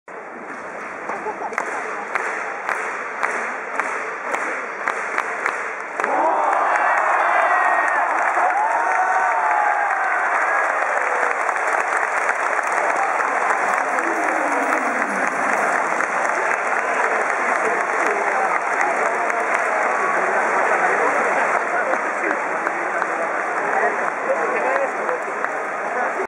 西武 栗山巧 タイムリーヒット 西武 vs ソフトバンク 2022年10月1日(土)
2022年10月1日(土)　西武 vs ソフトバンク　ベルーナドーム　現地で撮影
4回裏、1アウト満塁で栗山巧がセンター前にタイムリーヒット。